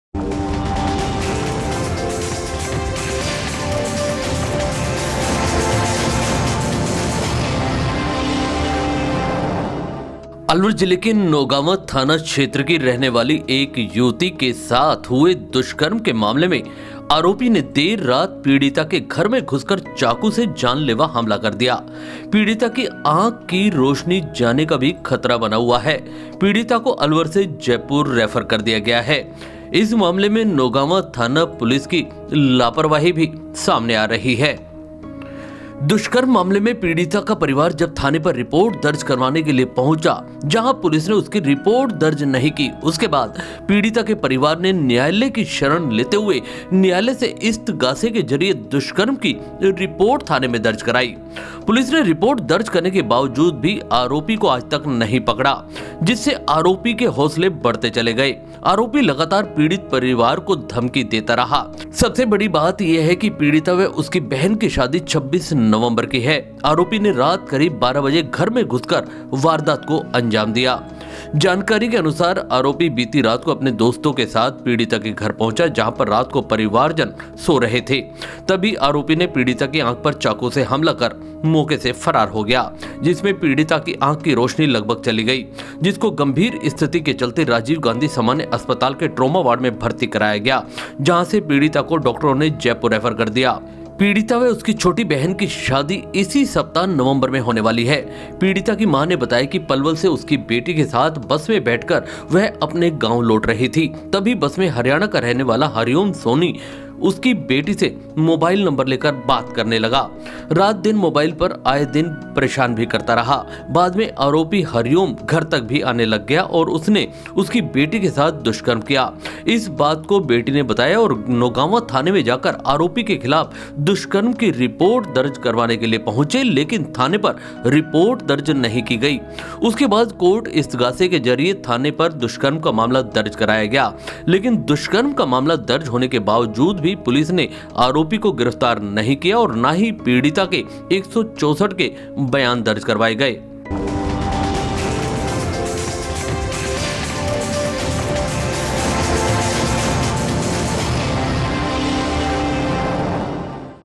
दुष्कर्म पीड़िता पर आरोपी ने किया जानलेवा हमला.
पीड़िता की आंख पर चाकू से हमला कर आरोपी हुआ फरार.
अलवर के नॉगांवा थाने की लापरवाही आई सामने.